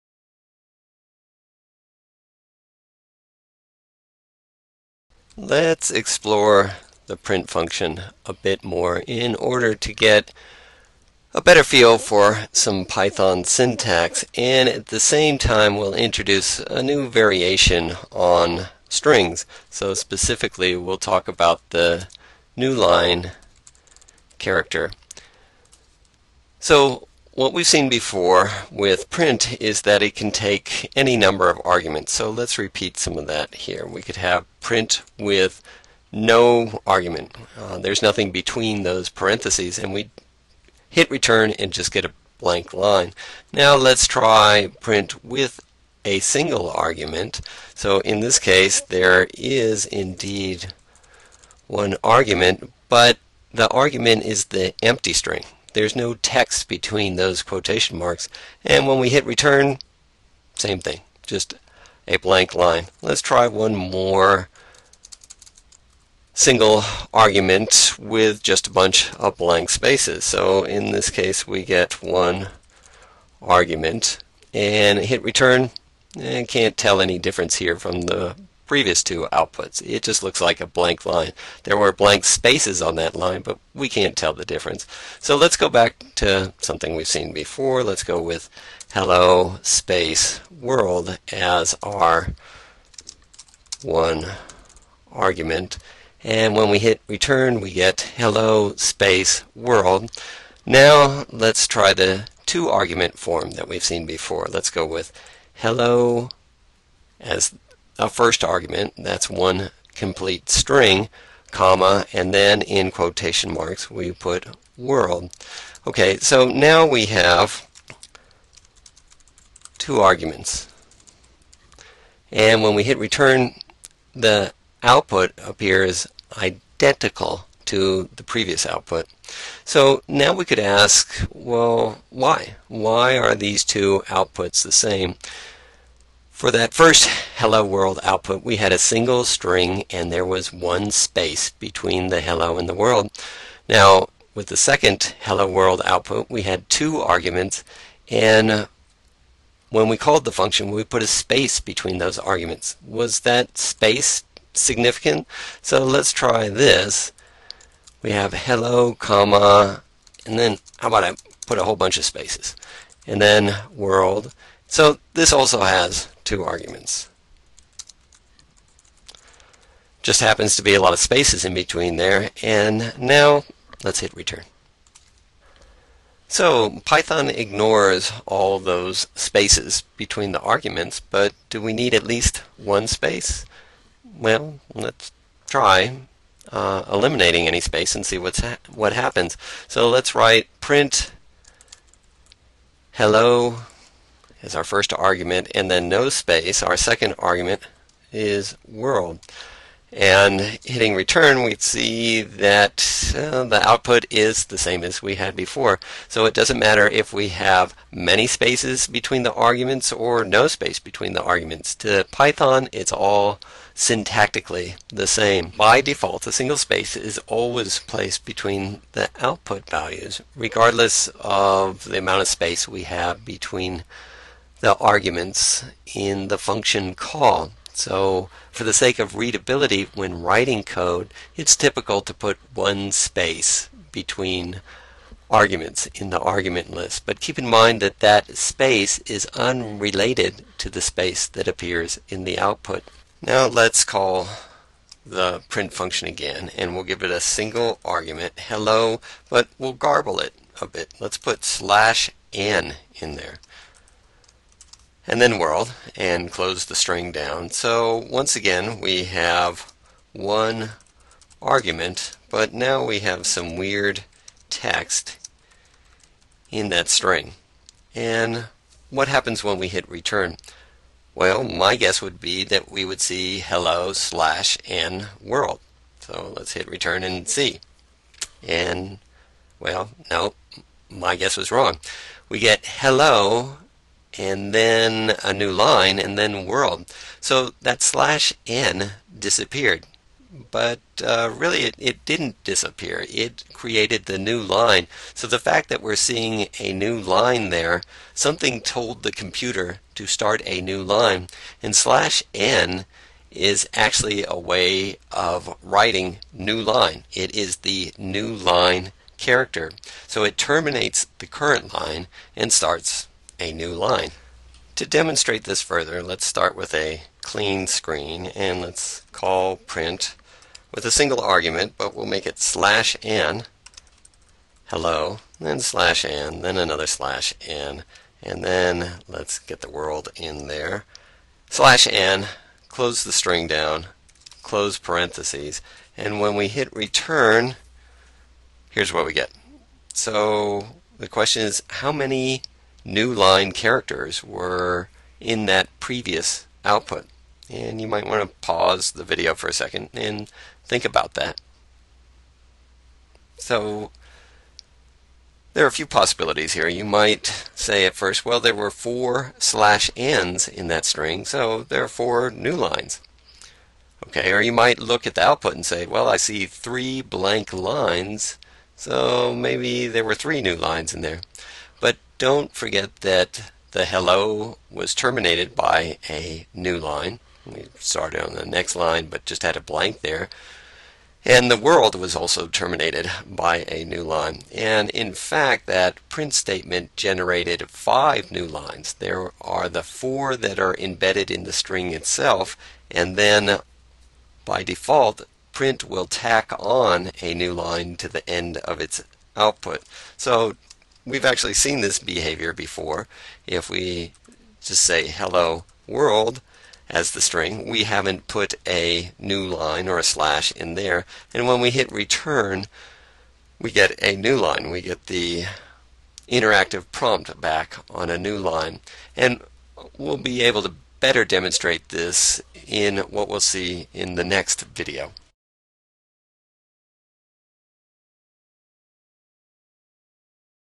Unit 1 Video 3: print() and the newline Character
Intro to Programming using Python
Instructor: John B. Schneider
Description: Further exploration of the print() function together with some syntax related to function arguments.  Also, a discussion of the newline character.